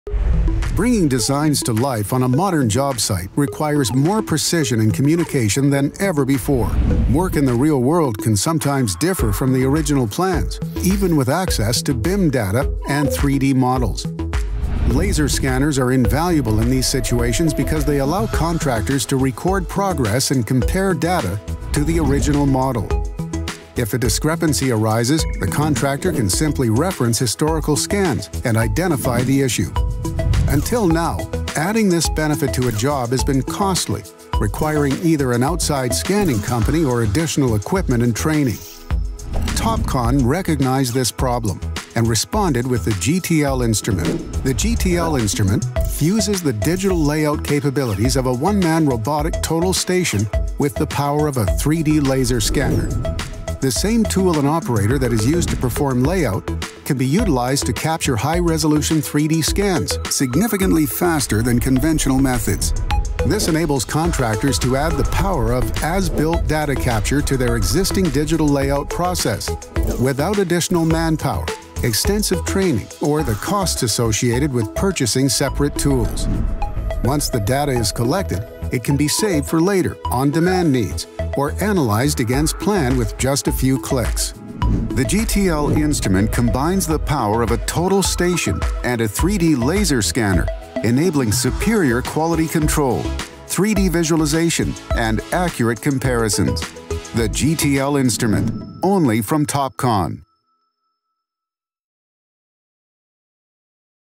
GTL Series | Vertical Construction Explained
The GTL Series combines the power of a robotic total station with a best-in-class laser scanner to perform digital layout and capture high-res 3D scans in a single setup. Simplify the scanning process and dramatically reduce the need for post-processing by capturing scan data on survey control. The workflow seamlessly integrates with ClearEdge3D software for advanced construction QA workflows and as-builts.